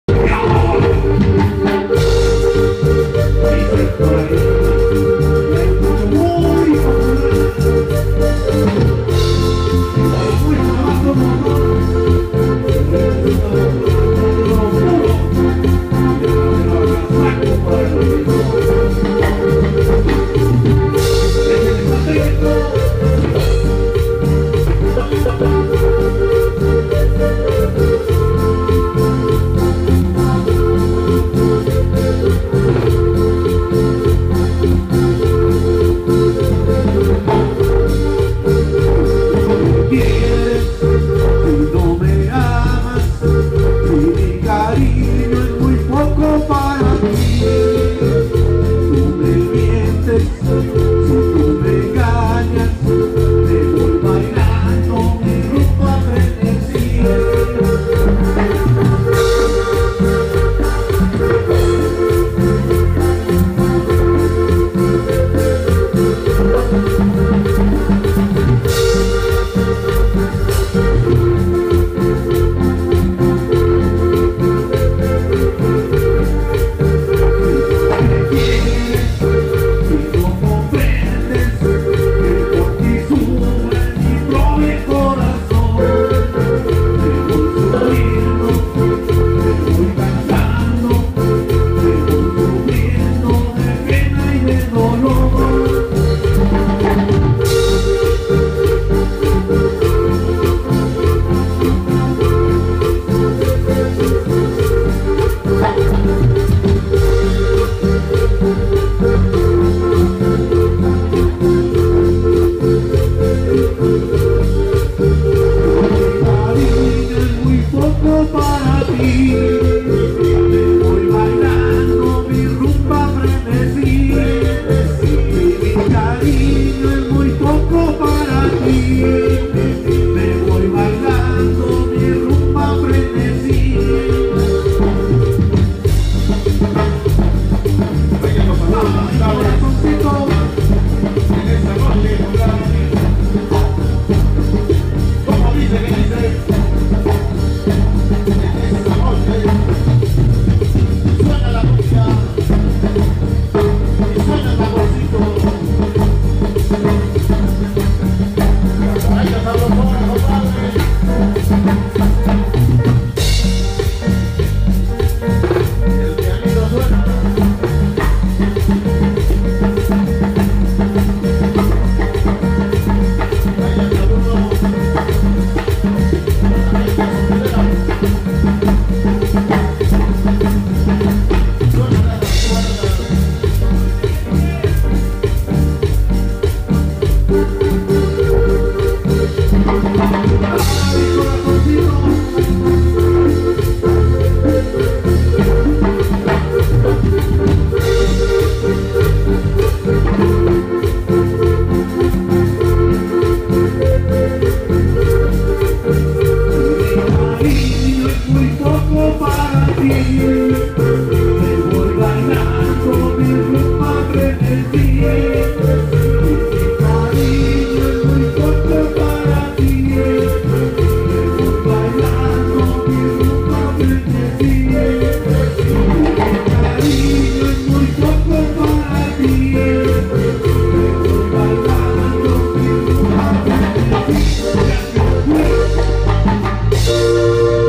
rumba frenezzi grupo magico musical en san andres cuexcontitlan 2017
Presentación en san andres cuexcontitlan toluca, estado de méxico conlindando con san diego de los padres.
musica sonidera  y ranchera